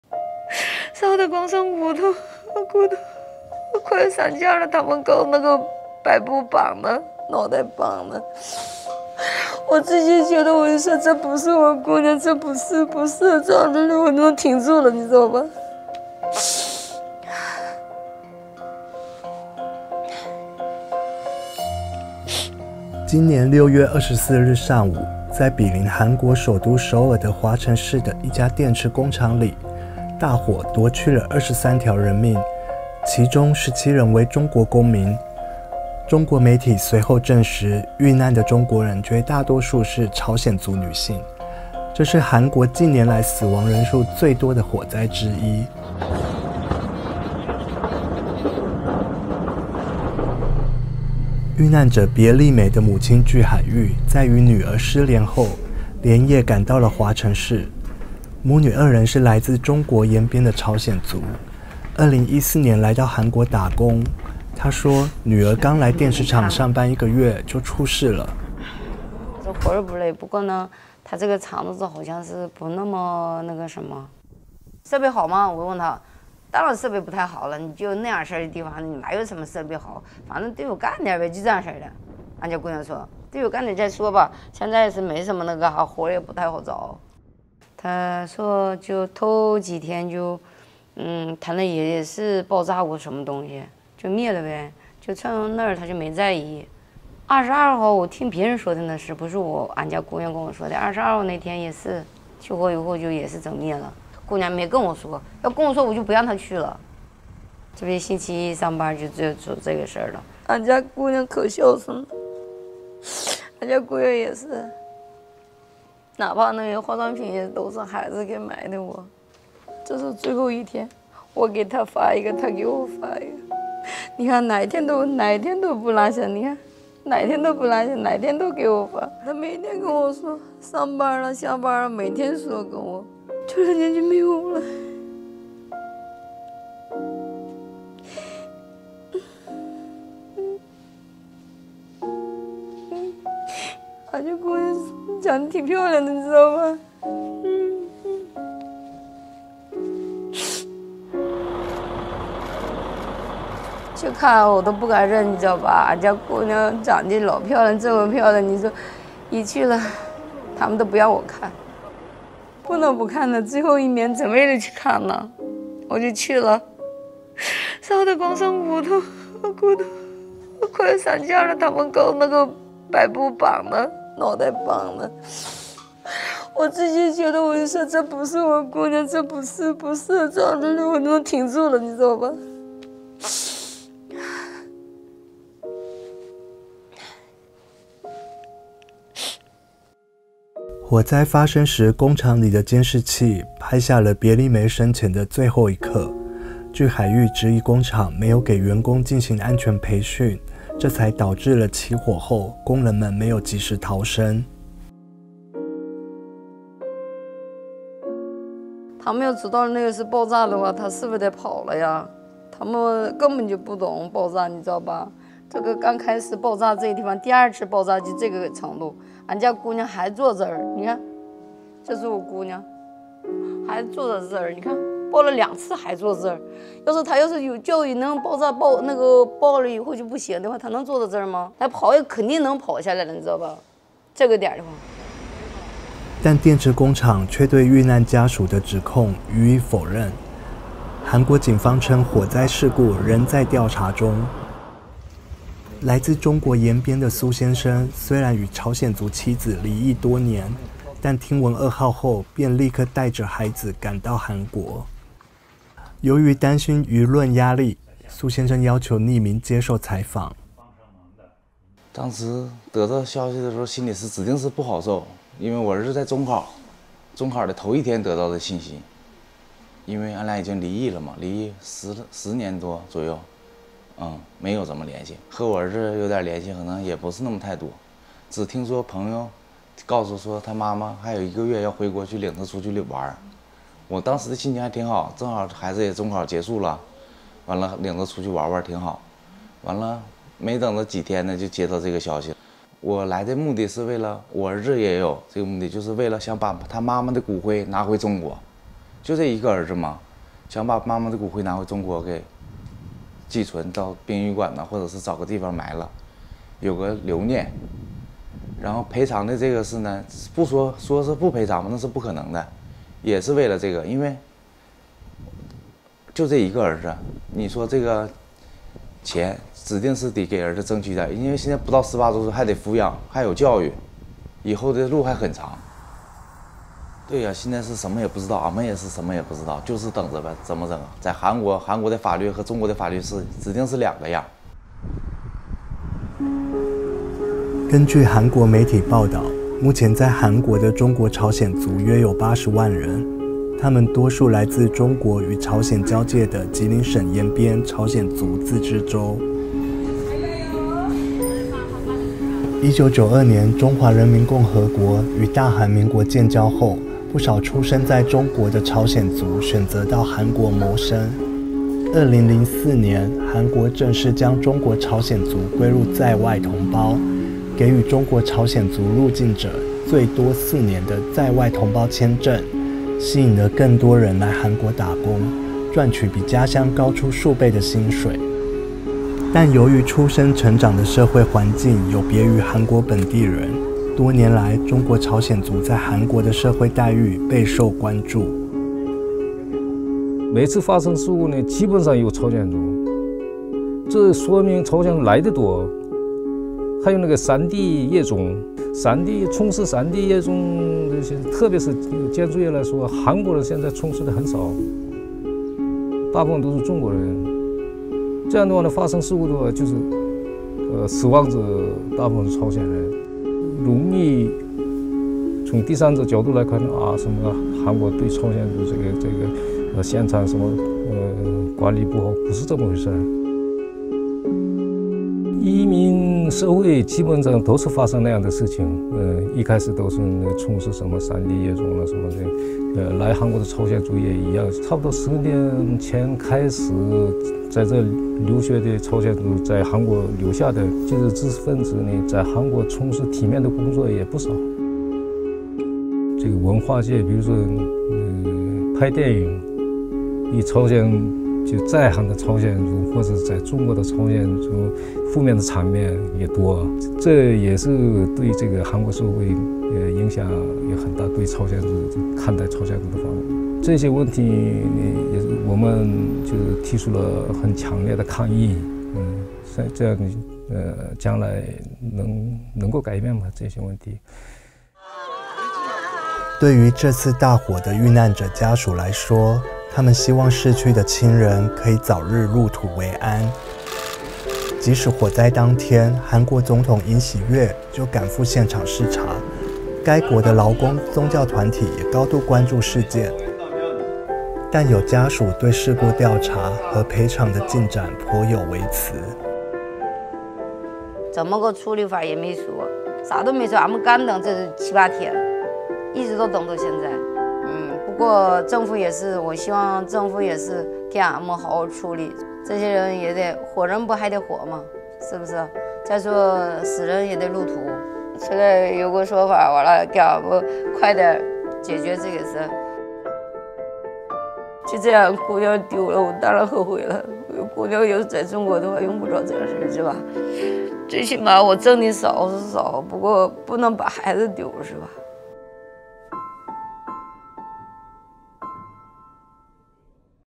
화성 아리셀 공장 화재 피해 유가족이 말하는 '조선족 노동자들' - BBC News 코리아
지난달 24일 발생한 화성 아리셀 참사로 23명이 숨졌다. 이 중 17명이 이른바 '조선족'으로 불리는 한국계 중국인으로 밝혀진 가운데, 중국 동포 유족들이 BBC에 심경을 전했다.

“차마 제 딸이라고 믿을 수 없었습니다. 너무나 예뻤던 딸이 뼛속까지 다 타버렸어요.”

주하이위 씨의 딸 비에리메(37) 씨는 공장에 다닌 지 한 달 만에 사고를 당했다. 이날 당시 촬영된 CCTV 화면엔 화재 발생 직후에도 작업 책상에 그대로 앉아 있는 비에리메의 모습이 그대로 담겨있다.

주 씨는 딸이 서둘러 대피해야 한다는 사실을 몰랐던 것 같다며 "(안전) 교육만 있었다면 이토록 젊고 건강한 청년들이 막다른 방향으로 달려갔겠냐"고 물었다.

주 씨는 "배상금도 상관없다"며 "딸이 죽은 이유, 딸을 위한 정의가 실현되길 바란다"고 말한다.

유족들은 현재 진상규명과 배상을 요구하고 있다. 한편 화성시는 10일 법적 근거가 없다는 이유로 아리셀 참사 유족들에 대한 숙식 지원을 중단하기로 했다. 이에 유족들은 시가 일방적으로 지원 중단을 통보했다며 반발하고 있다.

화성 아리셀 공장 화재 사고 유가족들과 시민단체들은 10일 기자회견을 열고 사고 책임자 5명을 고소 또는 고발했다고 밝혔다.

현재 국내에 거주 중인 중국 동포는 2023년 기준 약 65만 명. 한국 정부는 2002년부터 이들을 재외 동포로 공식적으로 인정해 법적 지위를 부여하기 시작했으며 2004년부터는 3년마다 연장이 가능한 '재외동포 비자'를 발급하고 있다.

취재: 뤼자홍, BBC 중국어 서비스
촬영·편집: 리뤄, BBC 중국어 서비스
자막: 이선욱, BBC 코리아